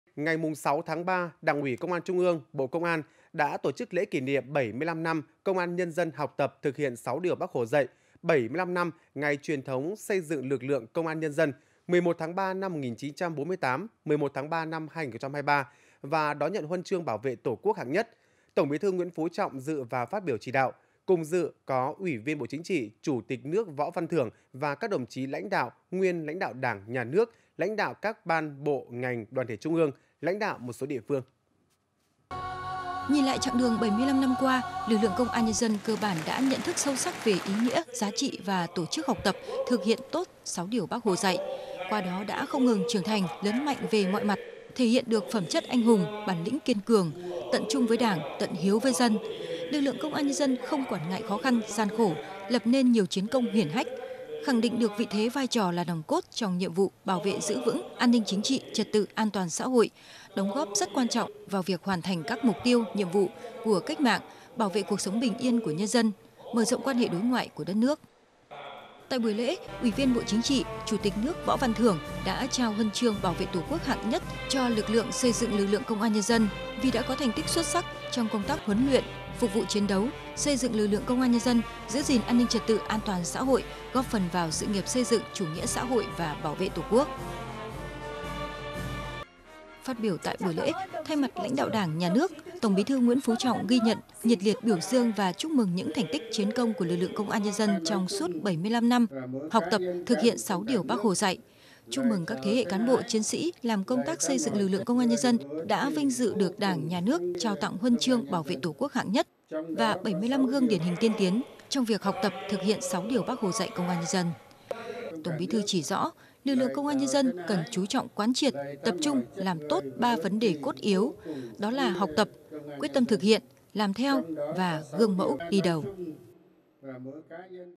Kỷ niệm 75 năm Công an nhân dân học tập, thực hiện Sáu điều Bác Hồ dạy
Ngày 06/3, Đảng ủy Công an Trung ương, Bộ Công an tổ chức Lễ kỷ niệm 75 năm Công an nhân dân học tập, thực hiện Sáu điều Bác Hồ dạy, 75 năm Ngày truyền thống xây dựng lực lượng Công an nhân dân (11/3/1948-11/3/2023) và đón nhận Huân chương Bảo vệ Tổ quốc hạng Nhất. Tổng Bí thư Nguyễn Phú Trọng dự và phát biểu chỉ đạo. Cùng dự có: Ủy viên Bộ Chính trị, Chủ tịch nước Võ Văn Thưởng và các đồng chí lãnh đạo, nguyên lãnh đạo Đảng, Nhà nước; lãnh đạo các ban, bộ, ngành, đoàn thể Trung ương; lãnh đạo Thành ủy, UBND thành phố Hà Nội, tỉnh Nghệ An, Bắc Giang, Quảng Ninh, Lạng Sơn, Bắc Ninh, Phú Thọ, Tuyên Quang.